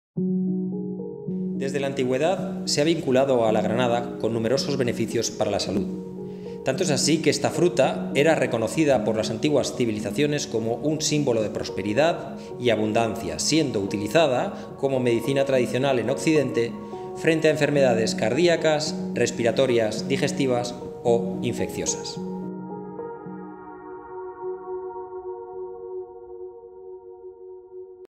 Curiosidades sobre la granada en la antigüedad
Las antiguas civilizaciones lo tenían claro, sabían que la granada poseía multitud de beneficios para la salud. Para ellos era un símbolo de prosperidad y abundancia y la utilizaban como medicina tradicional frente a enfermedades cardiacas, respiratorias, digestivas e infecciosas ✨

¡Desde entonces ya era considerada como un super alimento! ❤